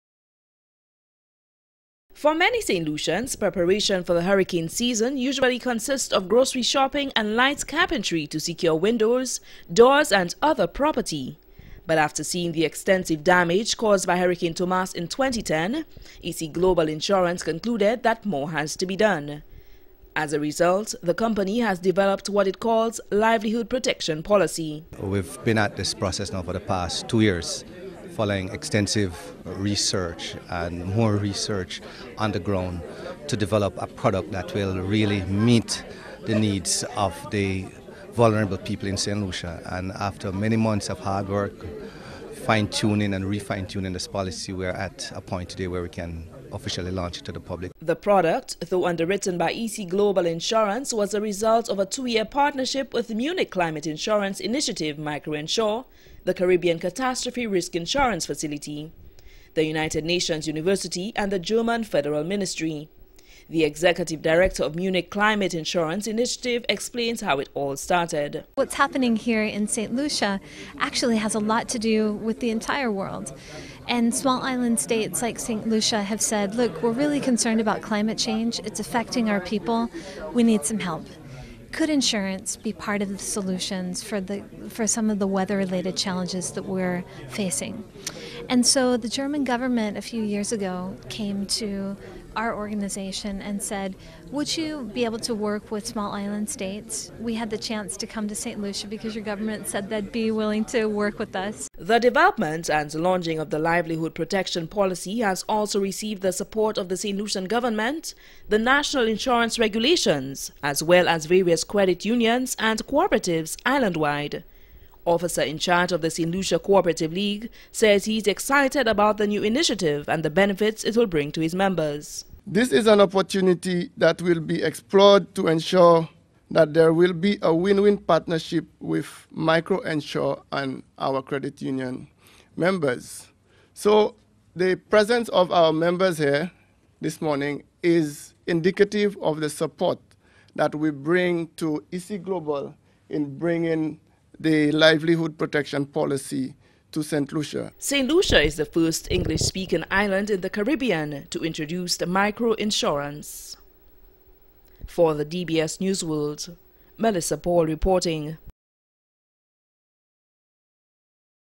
case of emergency
EC Global Insurance  is helping  low income earners financially prepare for the hurricane season.
The company has  partnered with the Munich(Mew-nick) Climate Insurance Initiative and the German government in the development and launching of a new  product aimed  to help low income earners prepare for the worst.
DBS's Melissa Paul has the details: